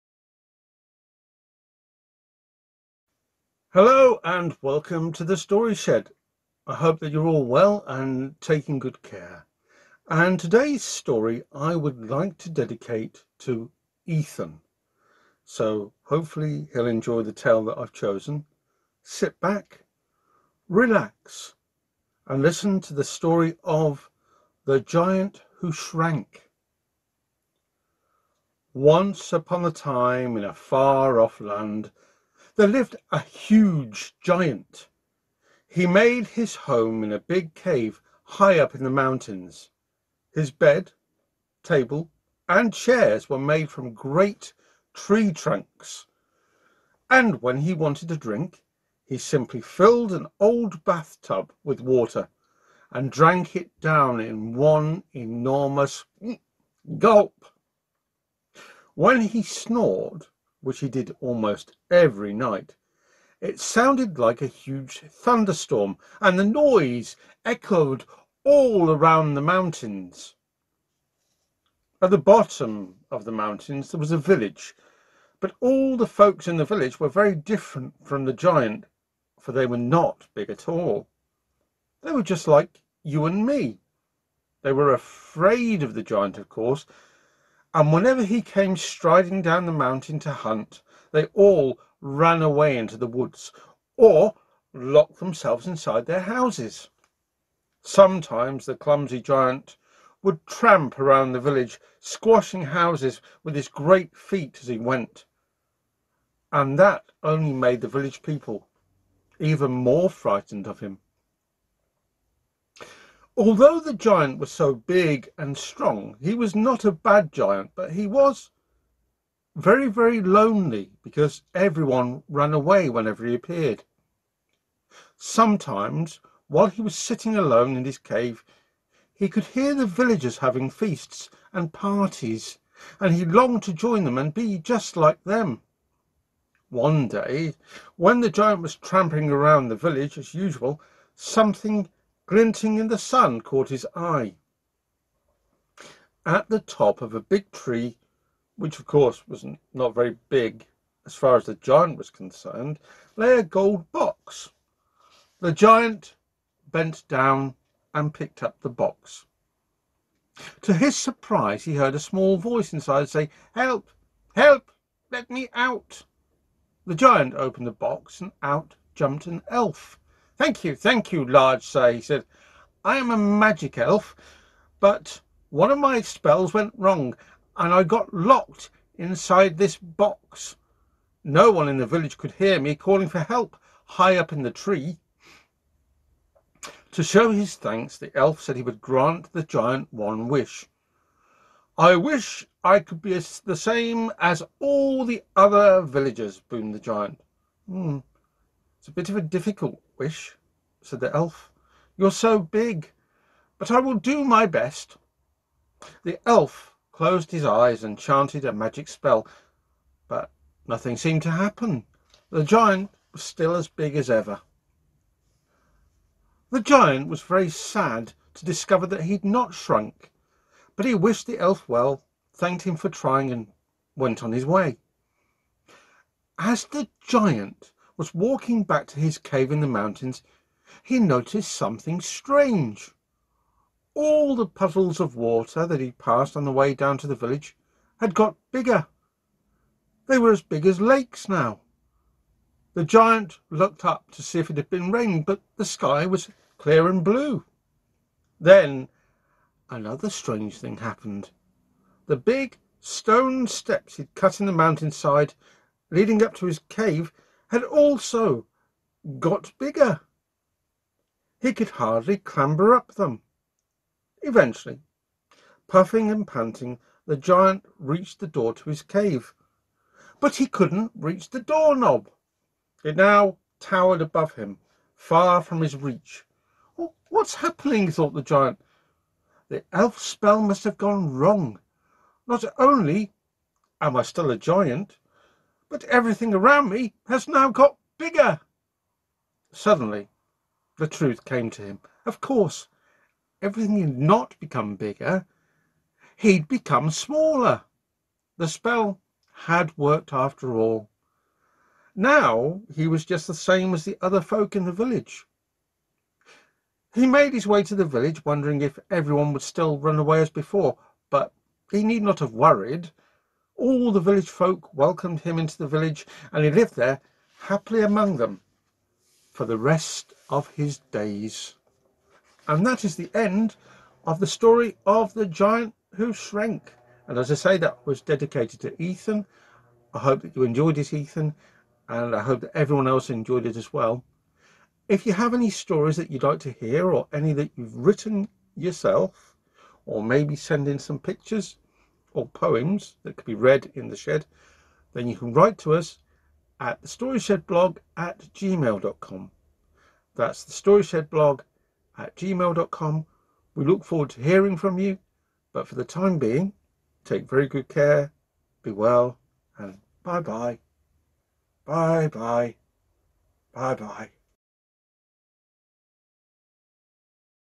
The Giant Who Shrank
A story about a giant who doesn't like being a giant. This story is dedicated to Ethan.